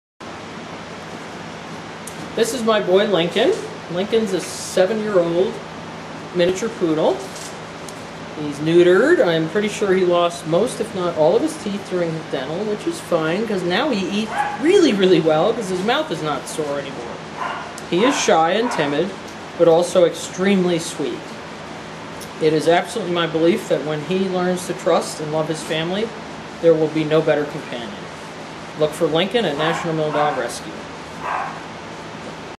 Meet Lincoln a Poodle Miniature currently available for adoption at Petango.com! 7/25/2014 3:43:52
Lincoln ~ Miniature Poodle, male, DOB 1/21/07.  Adoption fee $250.   Temperament update to come soon.  Learn about National Mill Dog Rescue